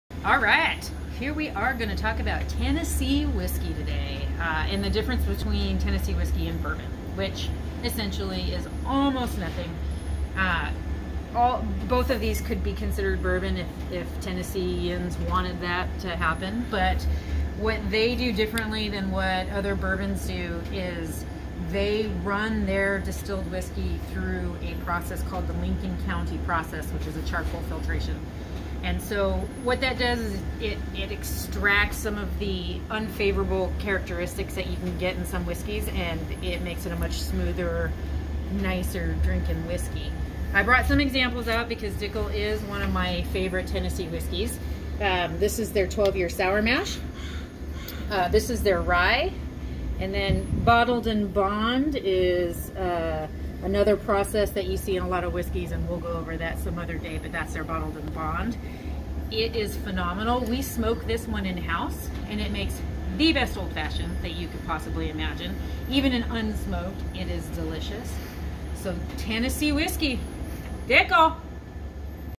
Meatless Monday: Dickel Tennessee Whiskey
MeatlessMonday: Dickel Tennessee Whiskey and the difference between bourbon and Tennessee Whiskey.